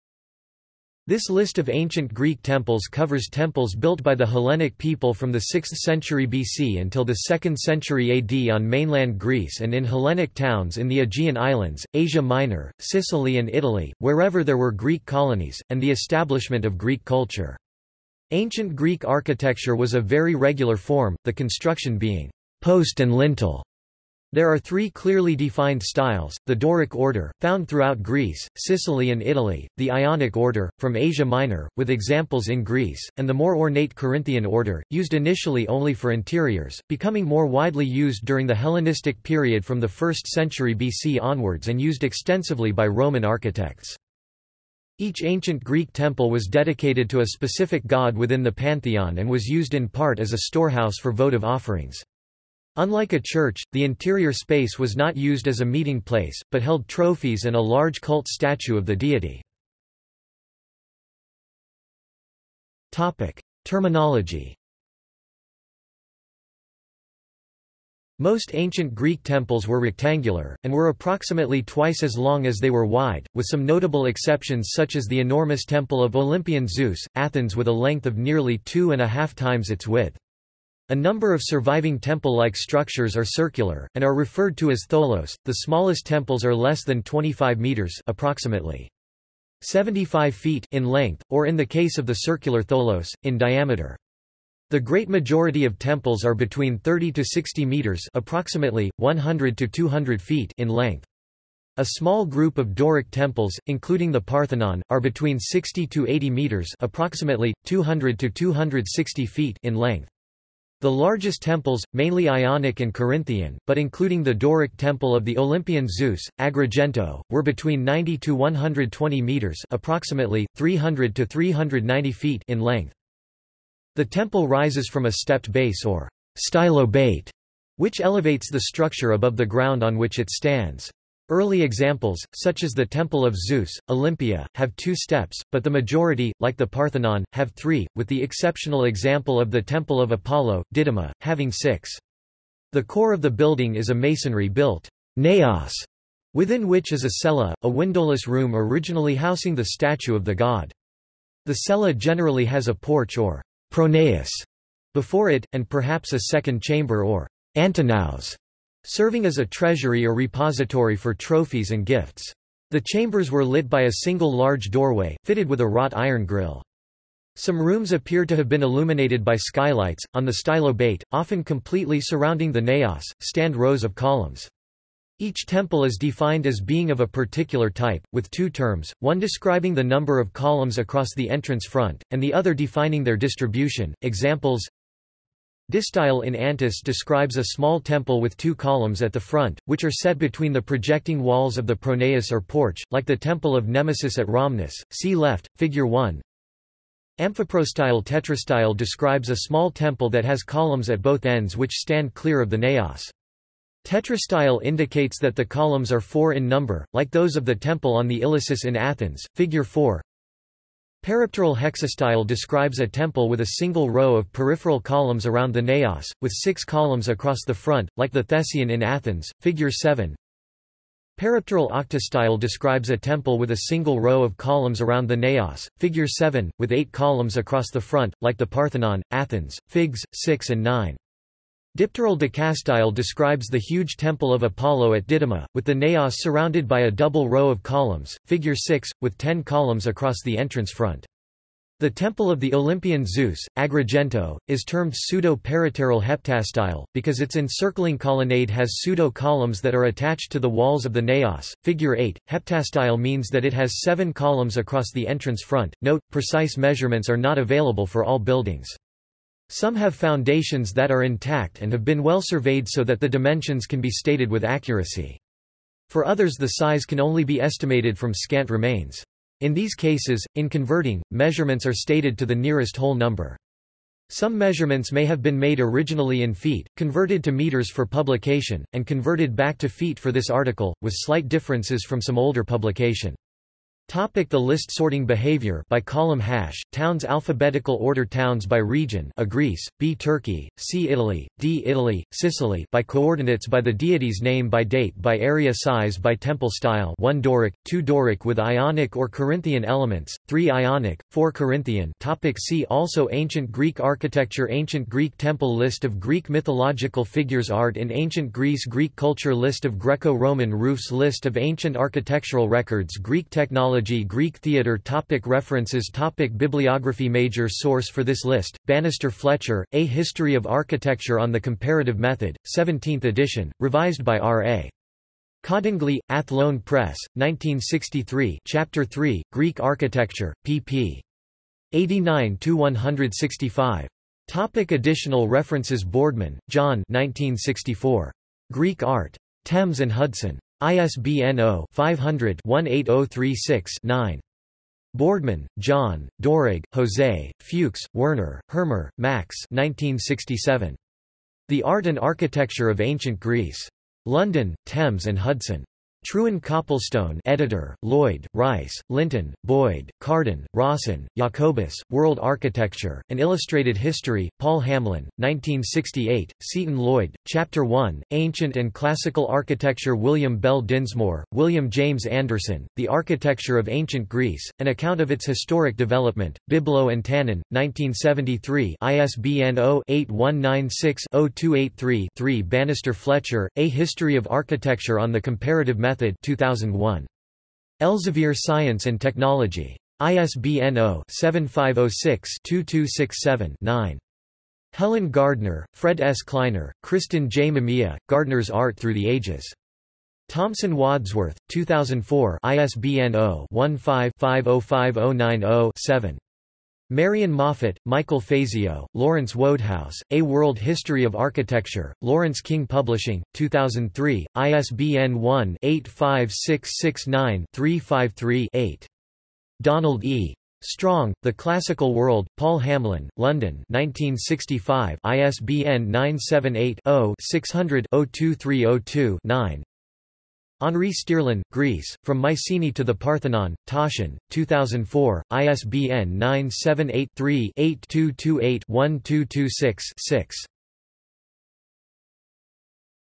List of Ancient Greek temples | Wikipedia audio article
This is an audio version of the Wikipedia Article:
List of Ancient Greek temples

- Socrates

SUMMARY
This list of ancient Greek temples  covers temples built by the Hellenic people from the 6th century BC until the 2nd century AD on mainland Greece and in Hellenic towns in the Aegean Islands, Asia Minor, Sicily and Italy, wherever there were Greek colonies, and the establishment of Greek culture. Ancient Greek architecture was of very regular form, the construction being "post and lintel". There are three clearly defined styles: the Doric Order, found throughout Greece, Sicily and Italy; the Ionic Order, from Asia Minor, with examples in Greece; and the more ornate Corinthian Order, used initially only for interiors, becoming more widely used during the Hellenistic period from the 1st century BC onwards and used extensively by Roman architects.
Each ancient Greek temple was dedicated to a specific god within the pantheon and was used in part as a storehouse for votive offerings. Unlike a church, the interior space was not used as a meeting place, but held trophies and a large cult statue of the deity.